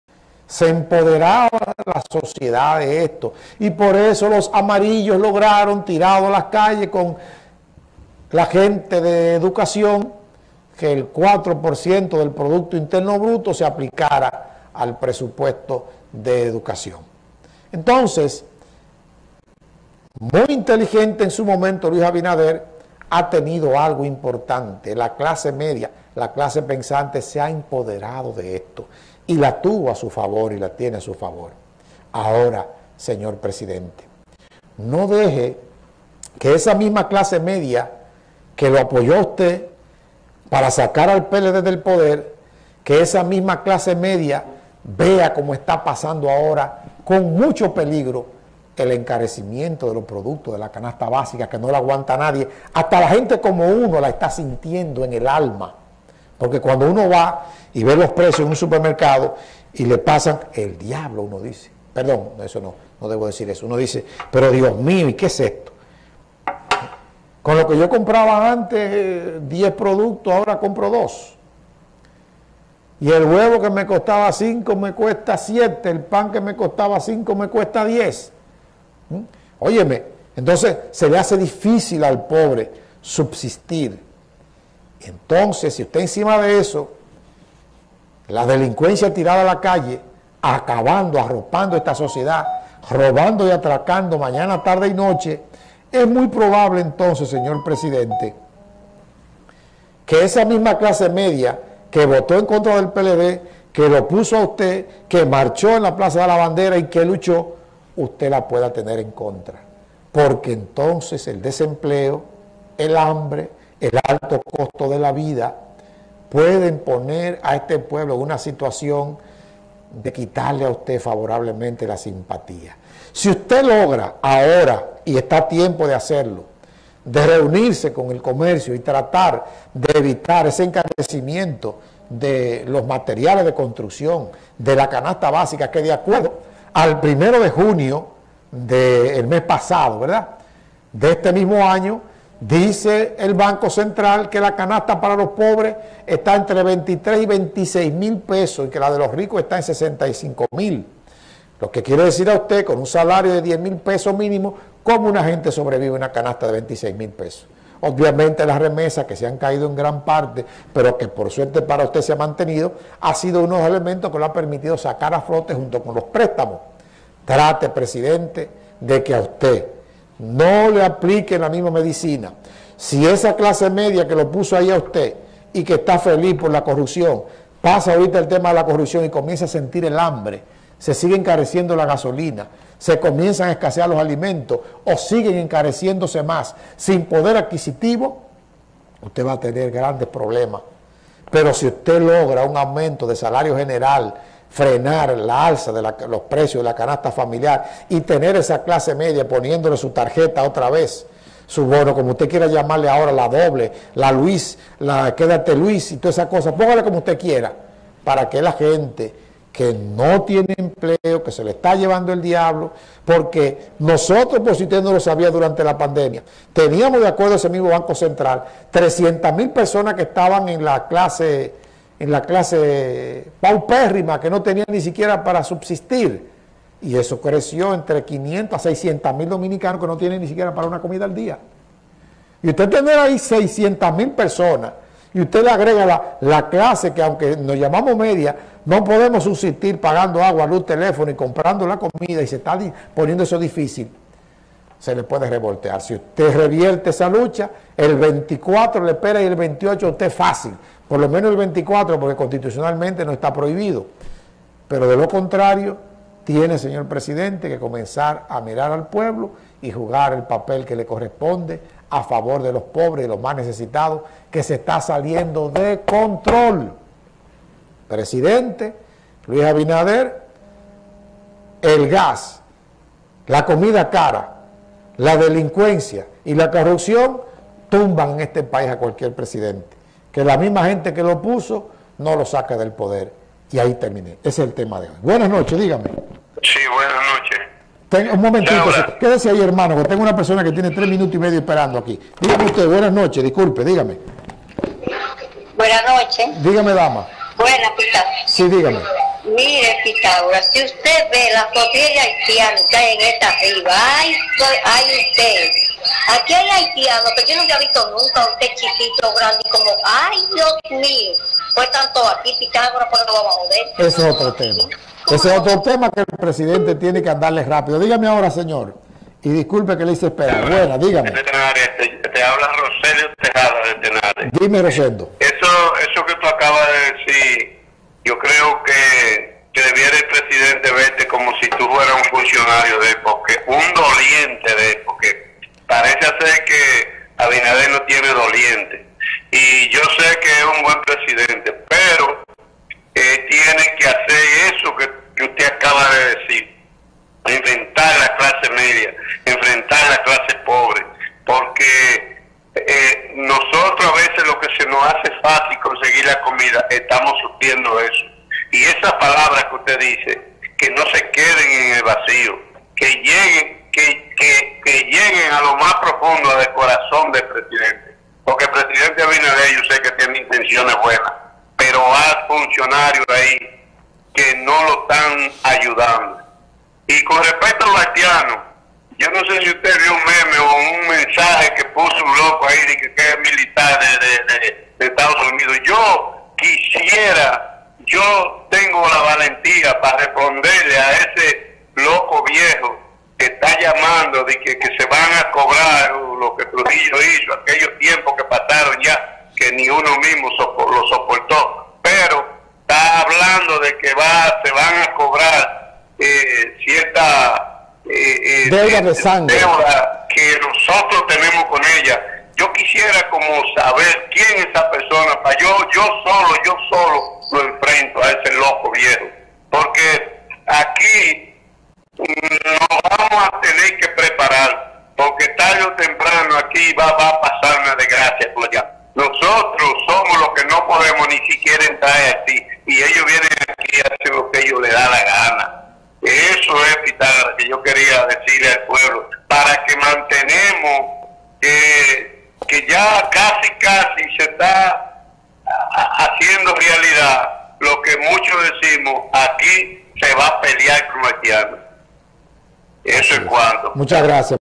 Los precios de la canasta básica , los combustible y la delicuencia pueden darle un golpe de estado
Los precios de la canasta básica , los combustible y la delicuencia pueden darle un golpe de estado económico al presidente